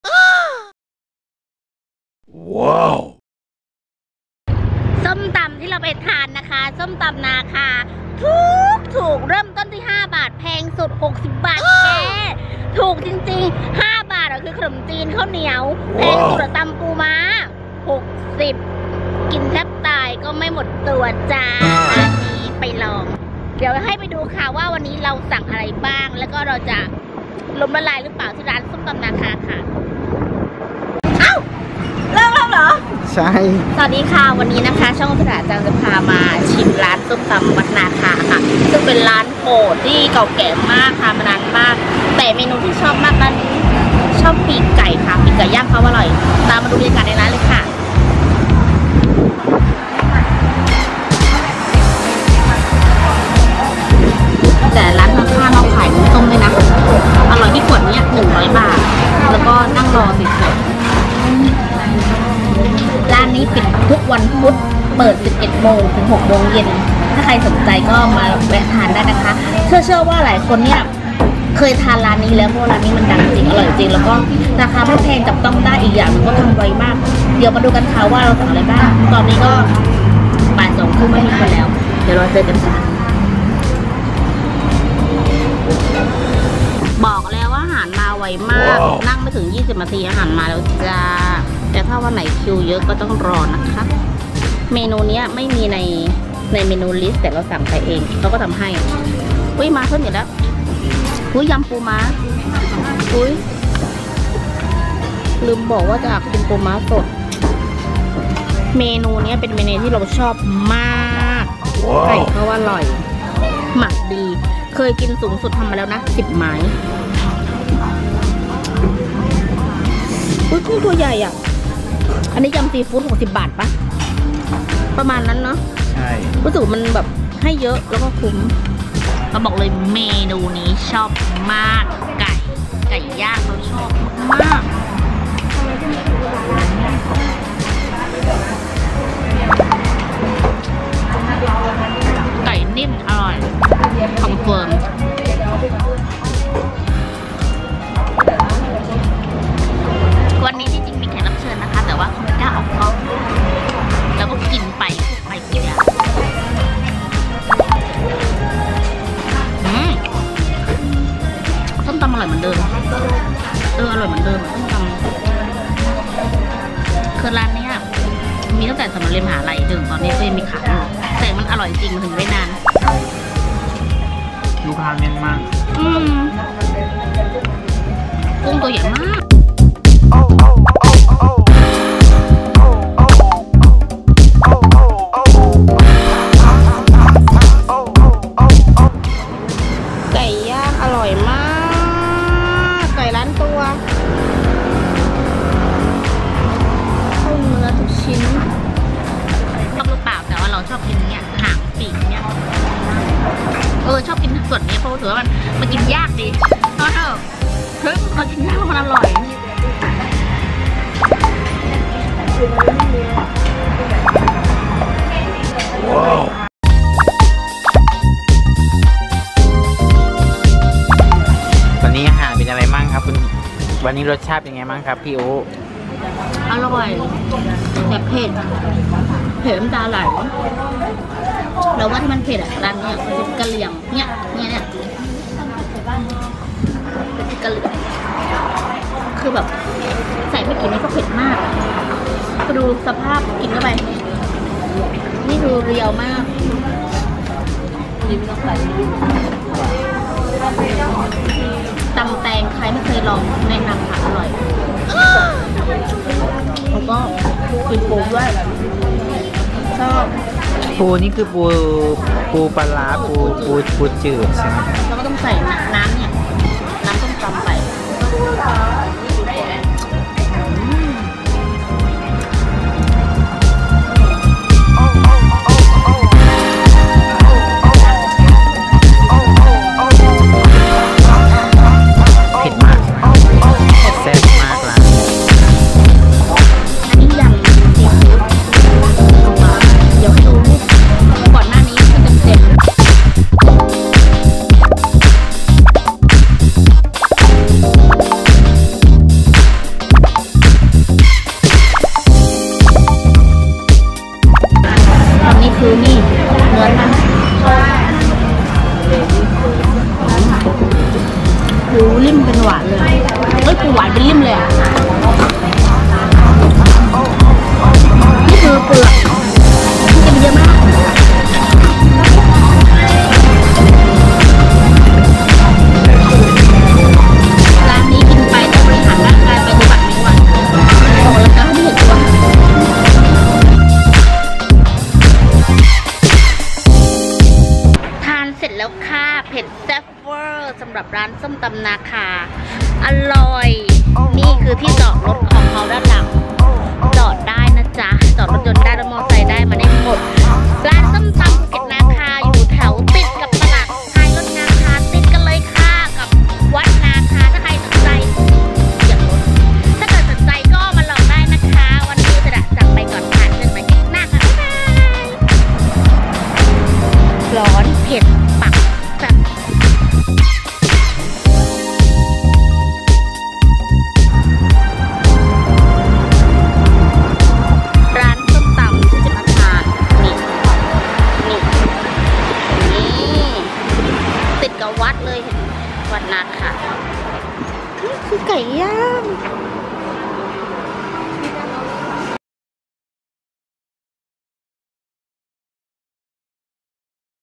ส้มตำที่ถูกที่สุดในภูเก็ต กำเงินไป 5 บาทก็มีขายค่ะ
ส้มตำวัดนาคา ตลาดท้ายรถภูเก็ตเค้าขายถูกมากกกกแกร  ส้มตำแทบทุกอย่างจานละ 30 บาท ข้าวเหนียว ขนมจีน 5 บาท แต่ไก่ย่างเค้าอร่อยมากกกกกกก  มากเป็นไก่ล้านตัวเลยค่ะ หมักเข้าเนื้อ หวาน ฉ่ำ เราเคยกินมาแล้ว 10 ไม้คนเดียว เพราะมันอร่อยมาก  ตำปูม้าก็ขาย 60 บาทเอง
แถวที่จอดรถยนต์และรอมอเตอร์ไซด์เพียบ ด้านหลังร้านเลยค่ะ
ถ้าใครยังนึกไม่ออกว่า ร้านอยู่ตรงไหน ถ้าเริ่มจากสี่แยกเซ็นทรัล ขับตรงไปทางฉลอง แล้วเลี้ยวซ้ายทีสามแยกไฟแดงแรก ตรงทางไปตลาดท้ายรถนาคา ร้านจะอยู่ฝั่งซ้ายมือ ก่อนถึงวัดนาคา ภูเก็ต